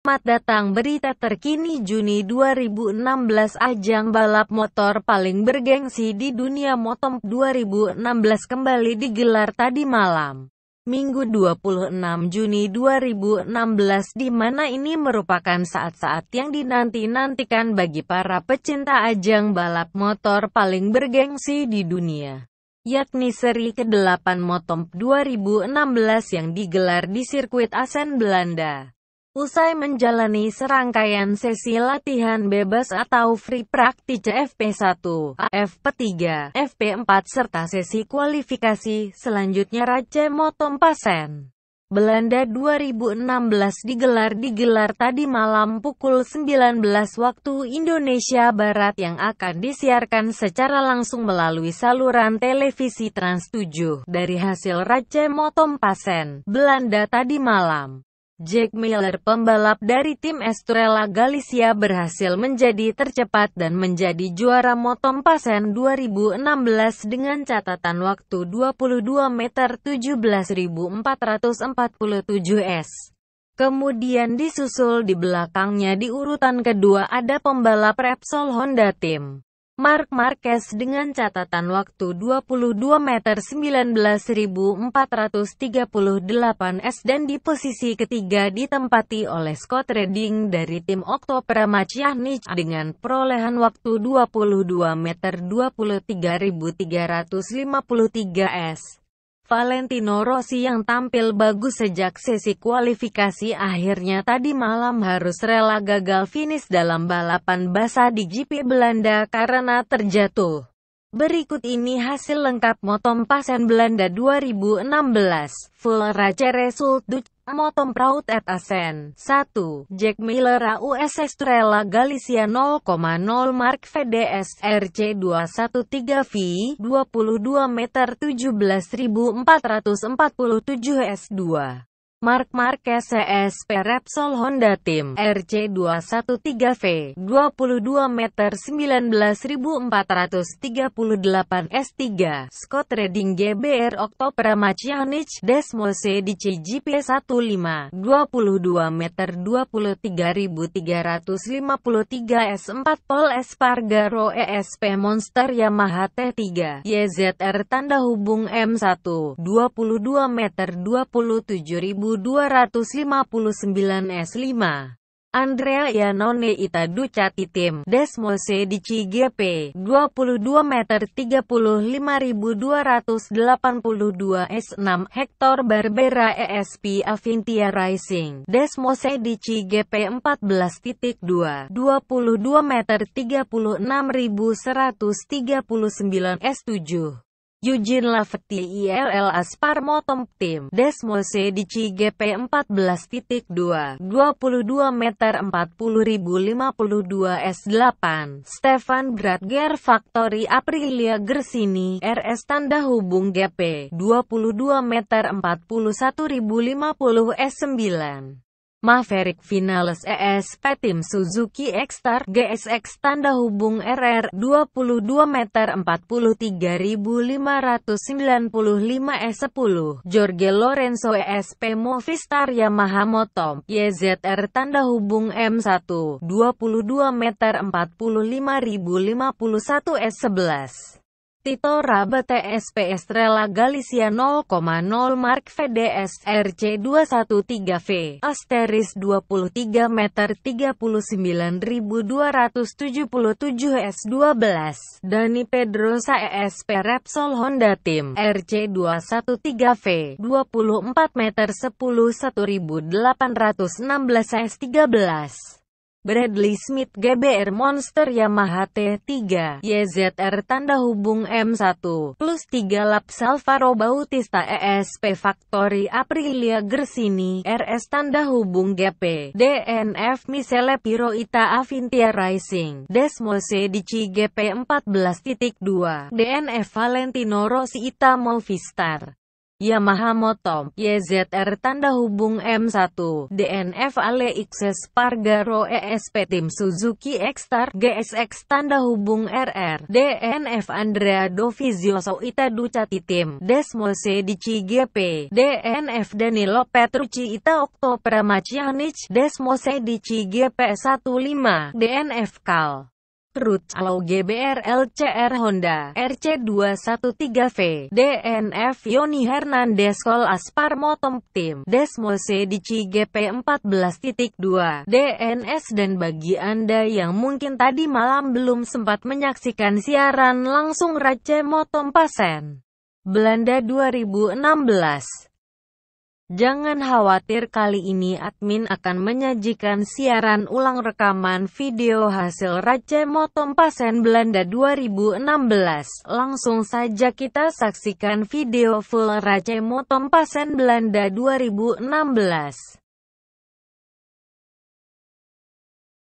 Video Full Race Hasil MotoGP Assen Belanda 2016
Ajang balap motor paling bergengsi didunia MotoGp 2016 kembali digelar tadi malam , Minggu 26 Juni 2016 dimana ini merupakan saat-saat yang dinanti-nantikan bagi para pecinta ajang balap motor paling bergengsi didunia , yakni seri ke-8 MotoGP 2016 yang digelar di sirkuit Assen Belanda . Usai menjalani serangkaian sesi latihan bebas atau Free Practice FP1,FP2,FP3,FP4 serta Sesi kualifikasi selanjutnya Race MotoGP Assen, Belanda 2016 digelar digelar tadi malam pukul 19.00 WIB yang akan disiarkan secara langsung melalui saluran televisi Trans7.

Dari Hasil race MotoGP Assen, Belanda tadi malam , Jack Miller pembalap dari Tim Estrella Galicia berhasil menjadi tercepat dan menjadi juara MotoGP Assen 2016 dengan catatan waktu 22m 17.447s , kemudian disusul dibelakangnya diurutan kedua ada pembalap Repsol Honda Team , Marc Marquez dengan catatan waktu 22m 19.438s dan diposisi ketiga ditempati oleh Scott Redding dari tim Octo Pramac Yakhnich dengan perolehan waktu 22m 23.353s. Valentino Rossi yang tampil bagus sejak sesi kualifikasi akhirnya tadi malam harus rela gagal finish dalam balapan basah di GP Belanda karena terjatuh. Berikut ini hasil lengkap MotoGp Assen Belanda 2016.

Full Race result Dutch MotoGP round at Assen.

1. Jack Miller AUS Estrella Galicia 0,0 Marc VDS (RC213V) 22m 17.447s
2. Marc Marquez ESP Repsol Honda Team (RC213V) 22m 19.438s
3. Scott Redding GBR Octo Pramac Yakhnich (Desmosedici GP15) 22m 23.353s
4. Pol Espargaro ESP Monster Yamaha Tech 3 (YZR-M1) 22m 27.259s
5. Andrea Iannone ITA Ducati Team (Desmosedici GP) 22m 35.282s
6. Hector Barbera ESP Avintia Racing (Desmosedici GP14.2) 22m 36.139s
7. Eugene Laverty IRL Aspar MotoGP Team (Desmosedici GP14.2) 22m 40.052s
8. Stefan Bradl GER Factory Aprilia Gresini (RS-GP) 22m 41.050s
9. Maverick Viñales ESP Team Suzuki Ecstar (GSX-RR) 22m 43.595s
10. Jorge Lorenzo ESP Movistar Yamaha MotoGP (YZR-M1) 22m 45.051s
11. Tito Rabat ESP Estrella Galicia 0,0 Marc VDS (RC213V)* 23m 39.277s
12. Dani Pedrosa ESP Repsol Honda Team (RC213V) 24m 11.816s
13. Bradley Smith GBR Monster Yamaha Tech 3 (YZR-M1) +3 laps
Alvaro Bautista ESP Factory Aprilia Gresini (RS-GP) DNF
Michele Pirro ITA Avintia Racing (Desmosedici GP14.2) DNF
Valentino Rossi ITA Movistar Yamaha MotoGP (YZR-M1) DNF
Aleix Espargaro ESP Team Suzuki Ecstar (GSX-RR) DNF
Andrea Dovizioso ITA Ducati Team (Desmosedici GP) DNF
Danilo Petrucci ITA Octo Pramac Yakhnich (Desmosedici GP15) DNF
Cal Crutchlow GBR LCR Honda (RC213V) DNF
Yonny Hernandez COL Aspar MotoGP Team (Desmosedici GP14.2) DNS

Dan bagi anda yang mungkin tadi malam belum sempat menyaksikan siaran langsung Race motoGP Assen Belanda 2016 , jangan khawatir kali ini admin akan menyajikan Siaran ulang rekaman Video Hasil Race MotoGP Assen Belanda 2016. Langsung saja kita saksikan Video Full Race MotoGP Assen Belanda 2016

Langganan di Channel ini terimakasih!